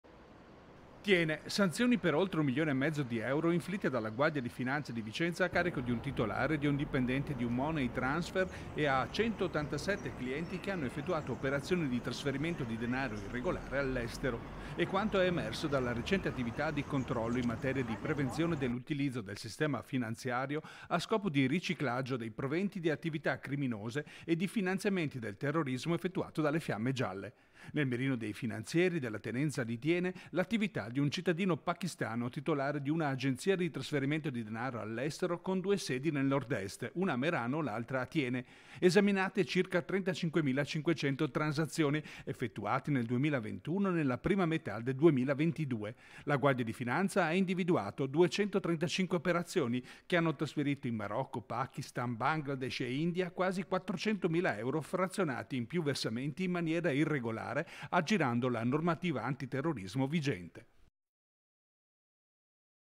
235 OPERAZIONI IRREGOLARI, 1.500.000 EURO DI SANZIONI A UN MONEY TRANSFER | 20/03/2023
TG Vicenza 20/03/2023 - THIENE - Controlli finanziari antiterrorismo della Guardia di Finanza a Thiene. Sanzionato un Money Transfer che ha effettuato 235 operazioni irregolari.  || THIENE Sanzioni per oltre un milione e mezzo di euro inflitte dalla Guardia di Finanza di Vicenza a carico di un titolare e di un dipendente di un Money Transfer e a 187 clienti che hanno effettuato operazioni di trasferimento di denaro irregolare all’estero. E’ quanto è emerso dalla recente attività di controllo in materia di prevenzione dell’utilizzo del sistema finanziario a scopo di riciclaggio dei proventi di attività criminose e di finanziamenti del terrorismo effettuato dalle Fiamme Gialle. Nel mirino dei finanzieri della tenenza di Thiene l’attività di un cittadino pakistano titolare di una agenzia di trasferimento di denaro all’estero con due sedi nel Nordest, una a Merano l’altra a Thiene. Esaminate circa 35.500 transazioni effettuate nel 2021 e nella prima metà del 2022. la Guardia di Finanza ha individuato 235 operazioni che hanno trasferito in Marocco , Pakistan, Bangladesh e India quasi 400 mila euro frazionati in più versamenti in maniera irregolare aggirando la normativa antiterrorismo vigente. (Servizio di Daniele Garbin) - Segui Rete Veneta anche sul digitale terrestre! Visita il sito